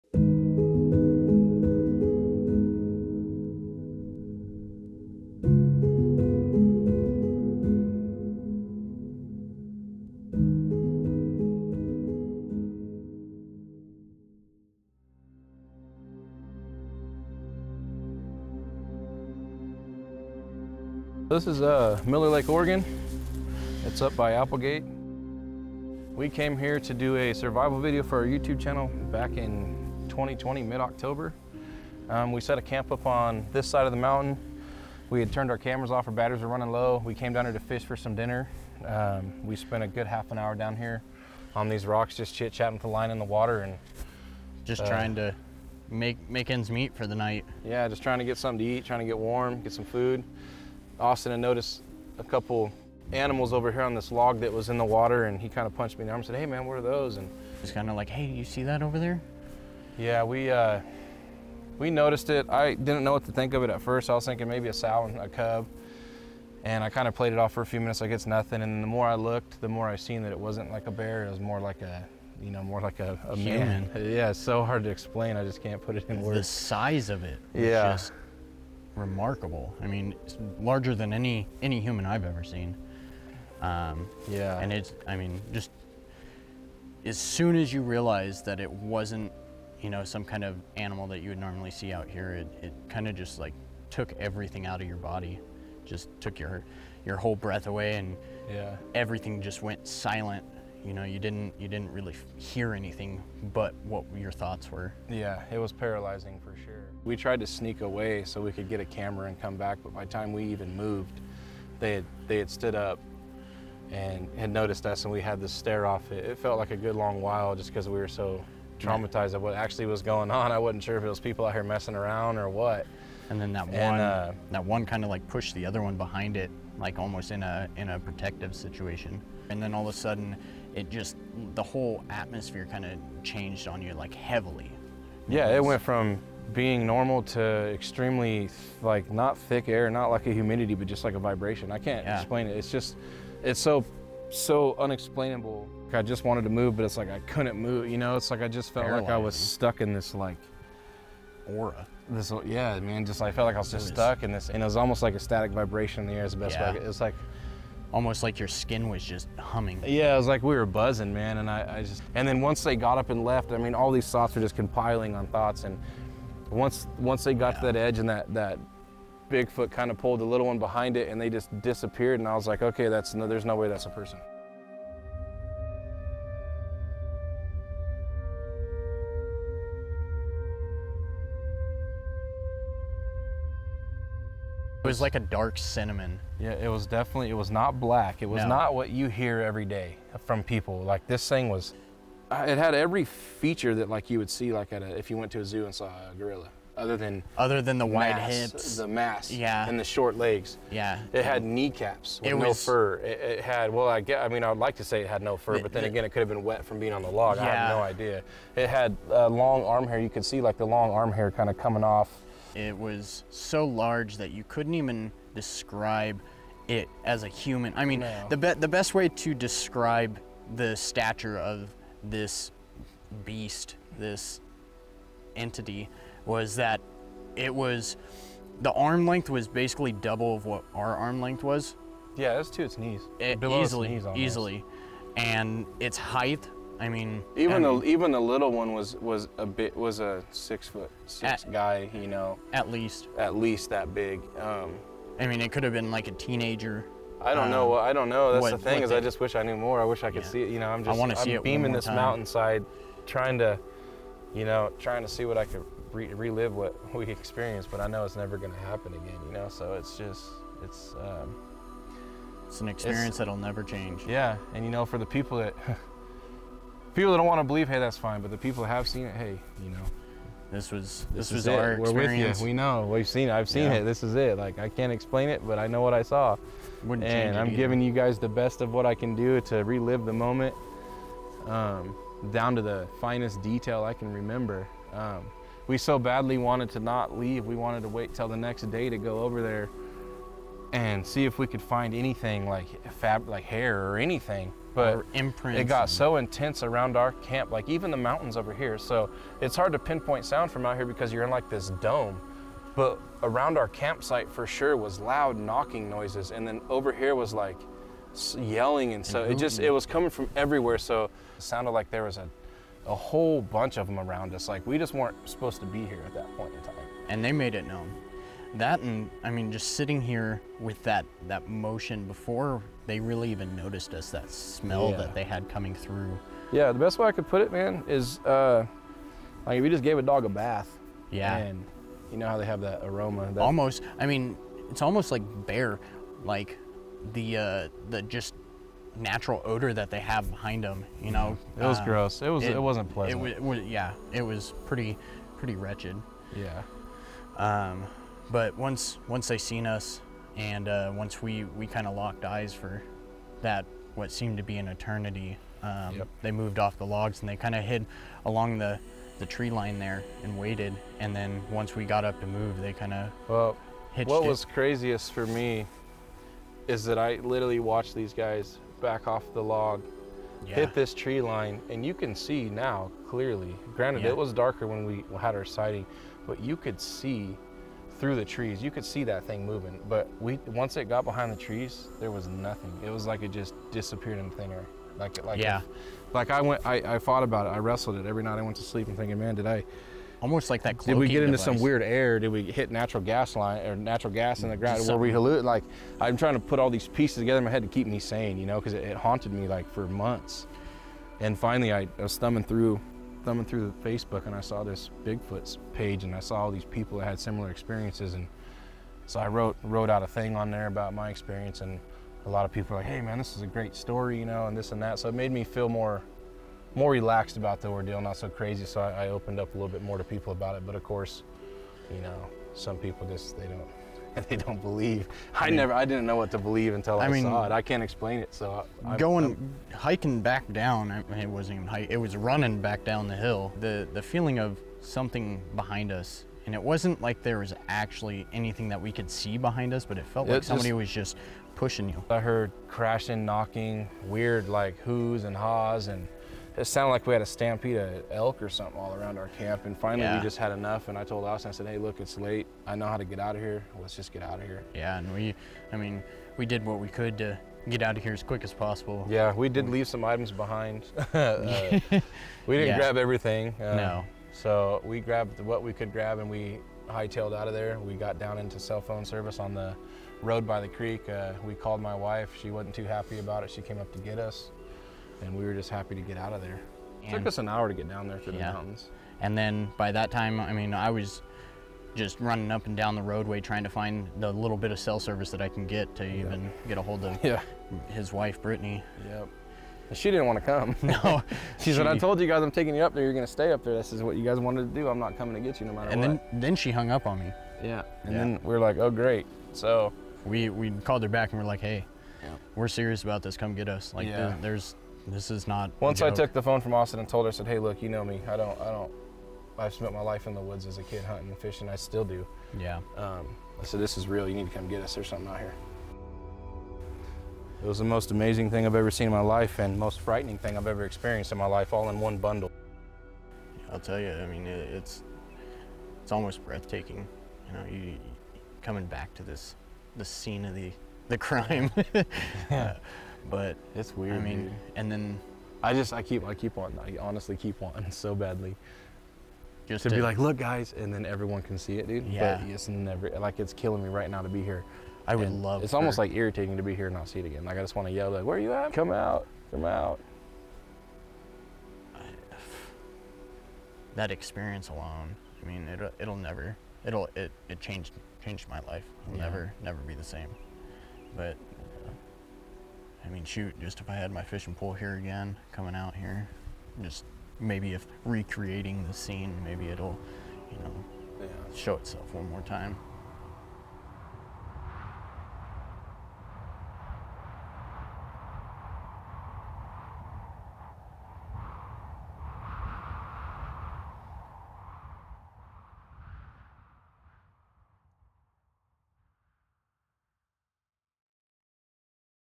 A Flash of Beauty: Chad and Austin on site
In May of 2021, we traveled to Miller Lake, Oregon (extreme southern Oregon on the border of California) with Chad and Austin to visit the exact spot where they had their sighting. After a grueling hike with camera gear (and some help from Chad with a motorbike), we reached the lake. Being there was like being watched. We knew the Bigfoot could be observing us at that very moment. Watch how Chad and Austin relive that fateful evening and see the range of emotions they experience as they recount a life-changing moment.